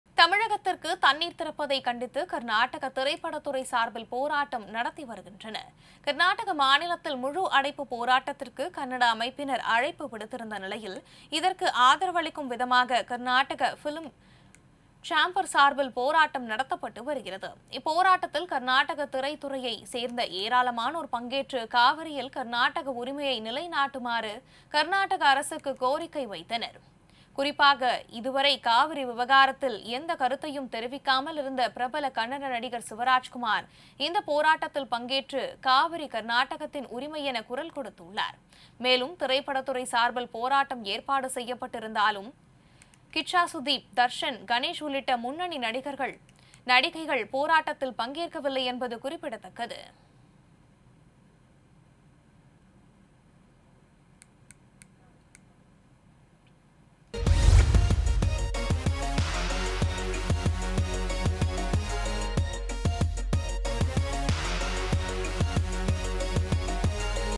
காவிரி தண்ணீருக்காக போராடிய திரைத்துறையினர்! | Karnataka Film stars | Cauvery protest
தமிழகத்திற்கு தண்ணீர் திறப்பதை கண்டித்து கர்நாடக திரைப்படத்துறை  சார்பில் போராட்டம் நடத்தி வருகின்றனர்.